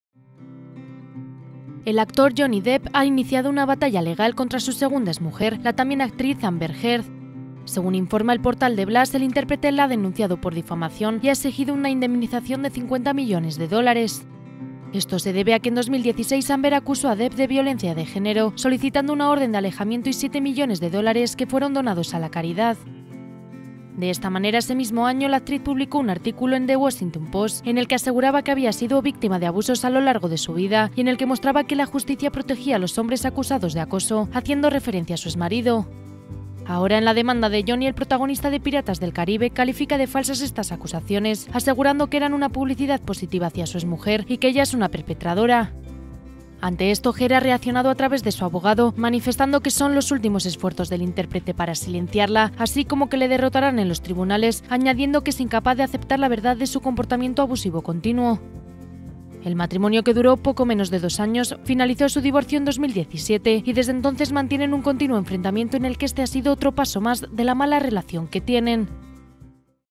Johnny Depp demanda a Amber Heard y exige 50 millones de dólares
El actor Johnny Depp ha iniciado una batalla legal contra su segunda exmujer, la tambien actriz Amber Heard. Según informa el portal 'The Blast', el intérprete la ha denunciado por difamación y ha exigido una indemnización de 50 millones de dólares.